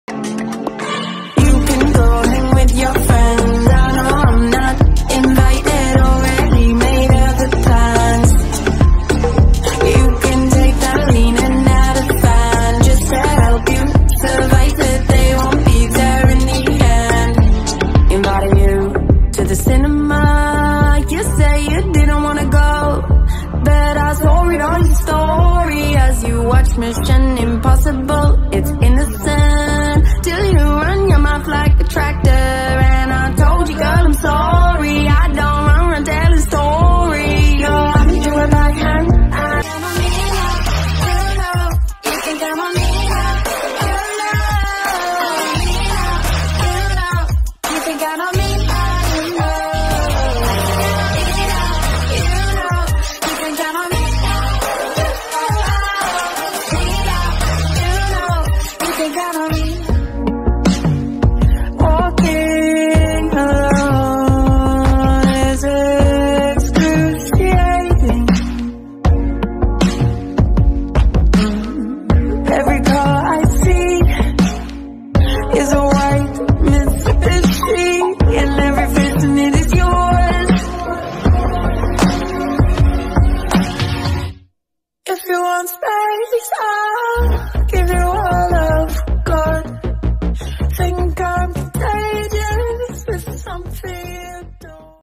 독특한 일렉트로닉 싸운드를 원한다면 주목!
달콤쌉싸름한 일렉트로닉! 싱송라 'Banoffee'